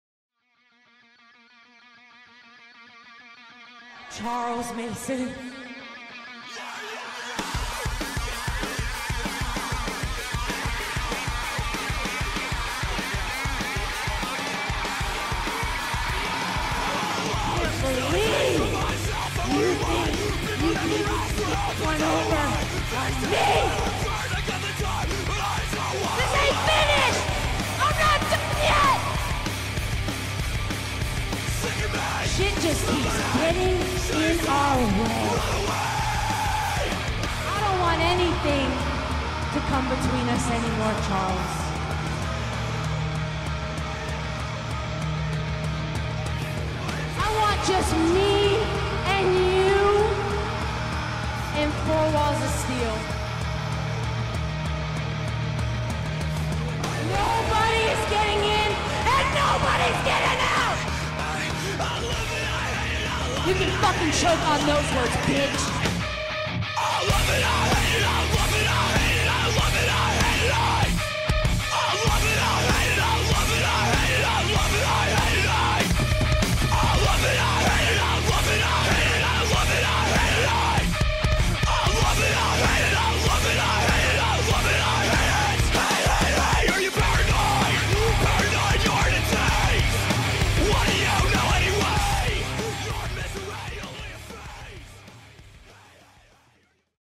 GCW - Charles Mason vs Allie Katch III Steel Cage Match | HYPE VIDEO
This Sunday!

Allie Katch and Charles Mason look to finish their bloody feud inside of a steel cage in The Showboat!